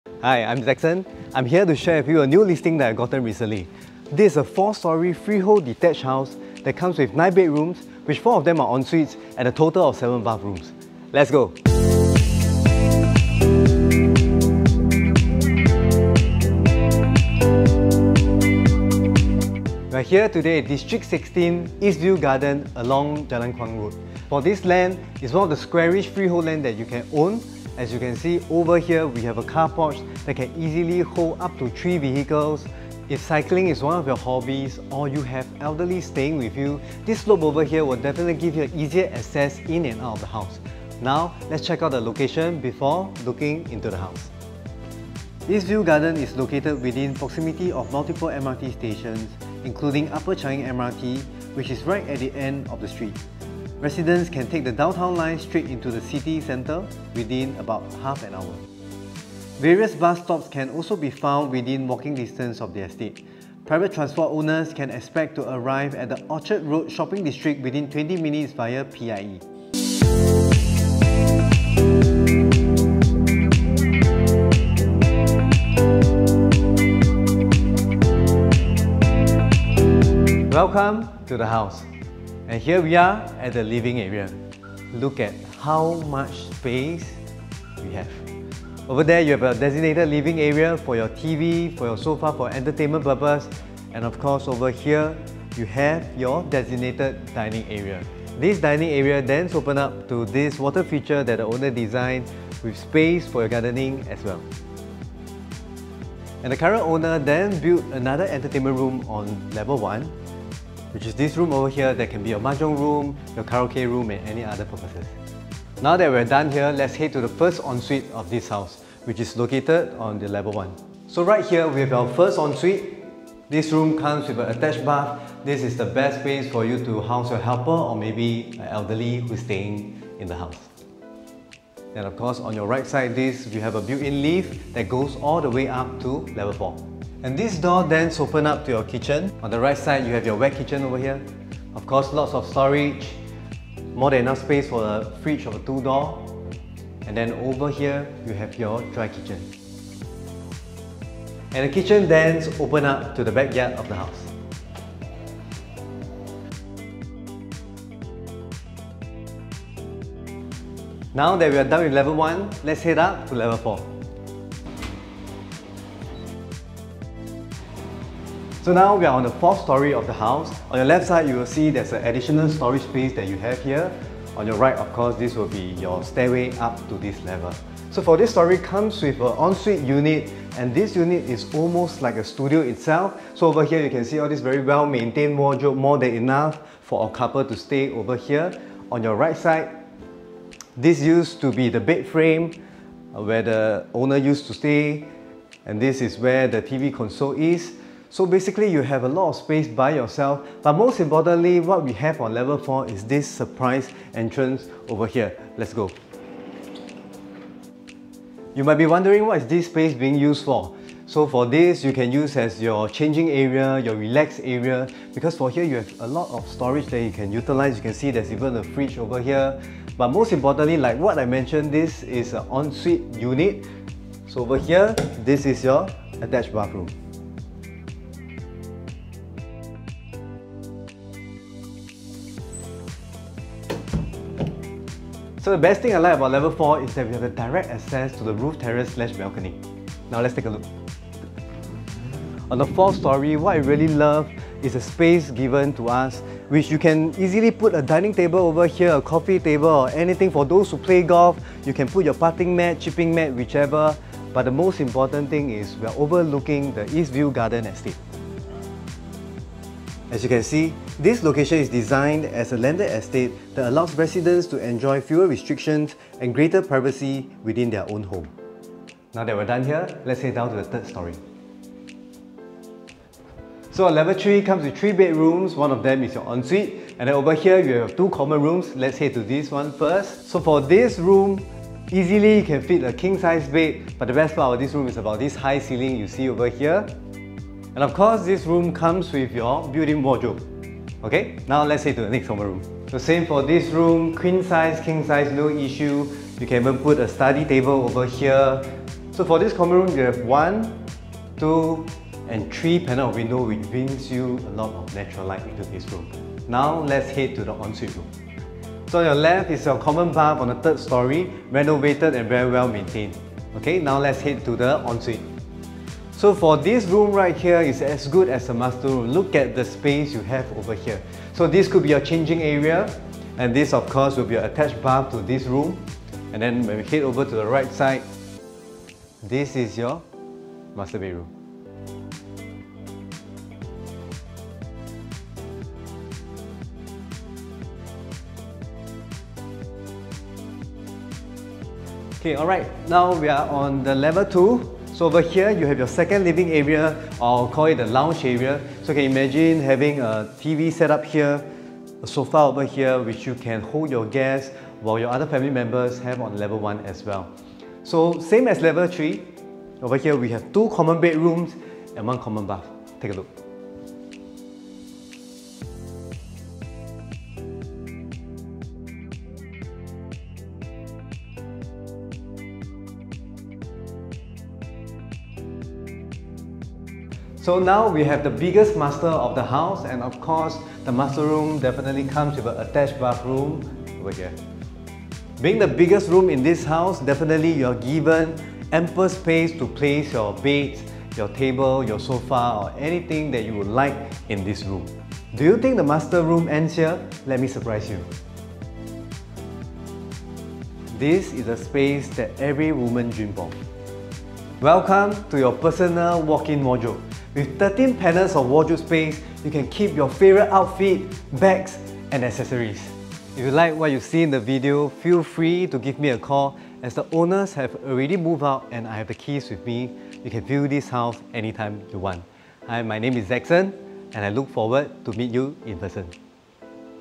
SOLD! Renovated Freehold 4-Storey Detached In District 16, East View Garden!
East View Garden is a terraced housing estate located along Meragi Terrace in District 16 It consists of around 70 units that are a mixture of freehold and 999-year leasehold properties. Units are available in 3 to 7-bedroom sizes. East View Garden is located within the Tampines planning area, a less-densely populated area on the outskirts of the city. Its location and design as a landed estate allows residents to enjoy fewer restrictions and greater privacy within their own home. The project is located within close proximity of all necessary amenities and services, and is also well-connected to both private and public transport links. The property is ideal for residents who wish to live in an independent, suburban neighbourhood conducive to a comfortable lifestyle.

East View Garden is located within proximity of multiple MRT stations, including Upper Changi MRT (Downtown Line), which is right at the end of the street. Residents can take the Downtown line straight into the city centre within about half an hour. Various bus stops can also be found within walking distance of the estate. Private transport owners can expect to arrive at the Orchard Road shopping district within 20 minutes via the Pan Island Expressway (PIE). Those heading to the business district can also take the PIE and expect to reach in around 30 minutes.

Source : PropertyGuru